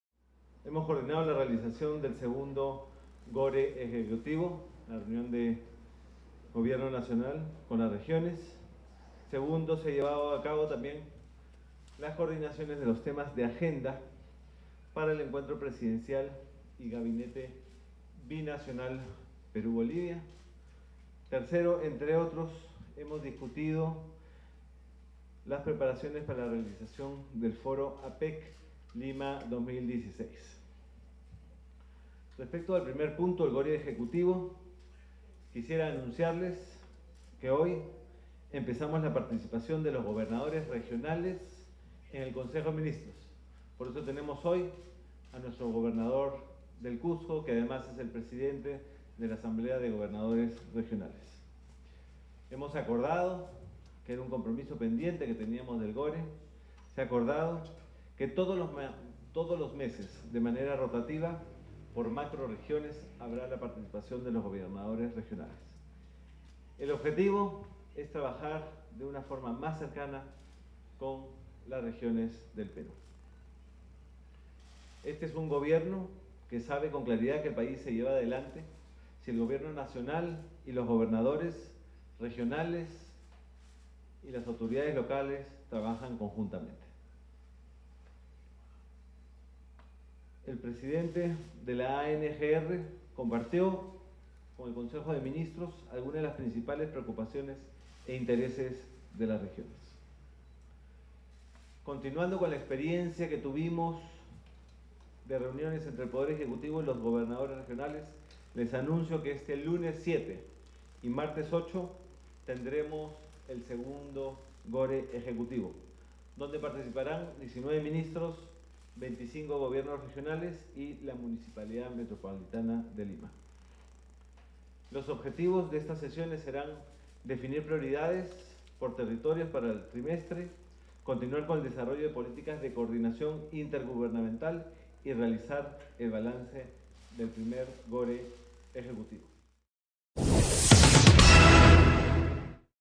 El 7 y 8 de noviembre se realizará segundo Gore Ejecutivo
Este lunes 7 y martes 8 de noviembre tendrá lugar en Lima el II Gore Ejecutivo, confirmó el presidente del Consejo de Ministros, Fernando Zavala.
En esta reunión participarán 19 ministros de Estado, 25 gobernadores regionales y la Municipalidad de Lima.